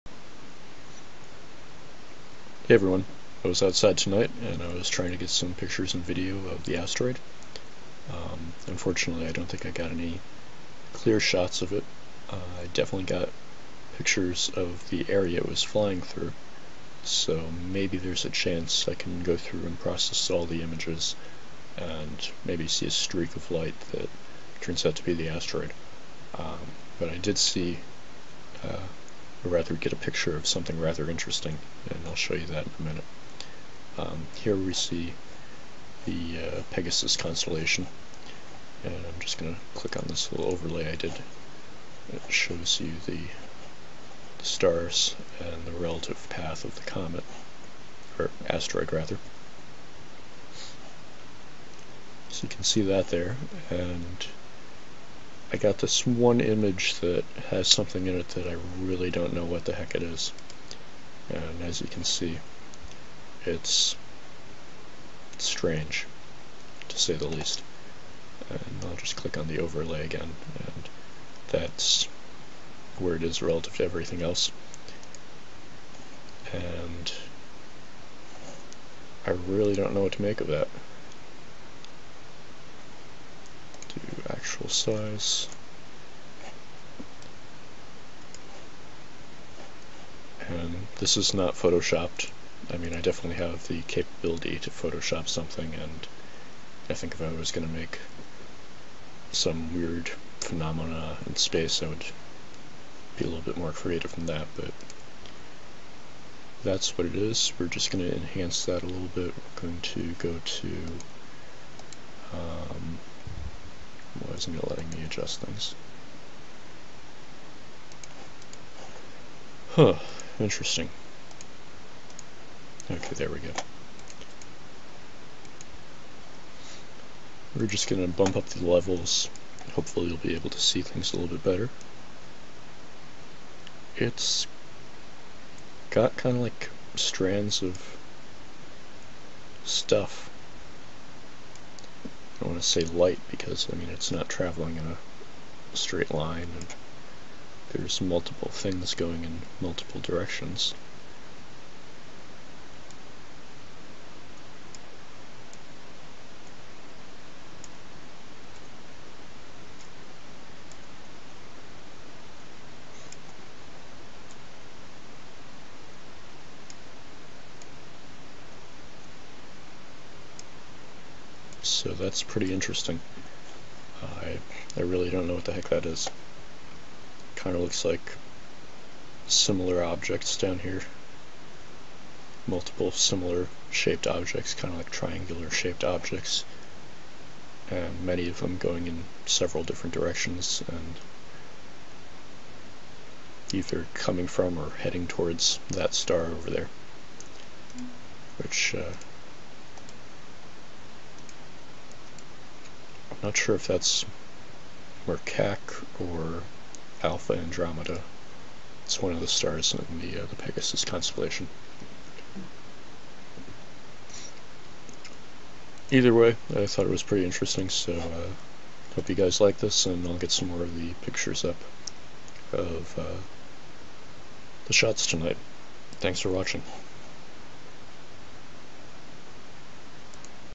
Alien Fleet Photographed Near Asteroid YU55 NOV 8 8:39 PM
I do a good bit of stellar photography and this looked really unusual.

If you're not familiar with taking long exposures of stars, you will get streaks of light, as you can see all the stars are arc shaped streaks, because the camera is stationary and the earth is moving.

The objects however are not streaked, which would seem to mean that they were in a stationary orbit moving with the earth, until traveling away at great speed.

The pictures were taken with a Canon T3i Digital Rebel 18 MP DSLR with  18-55 mm f 3.5 lens and  50 mm F1.4 lens

The picture with the "alien fleet" in it was taken at F1.4 with a low ISO and a 30 second exposure.

What do you think it is?

Space ships studying the passing asteroid? Some strange natural event? An Alien armada?

Nov 8  8:39 PM EST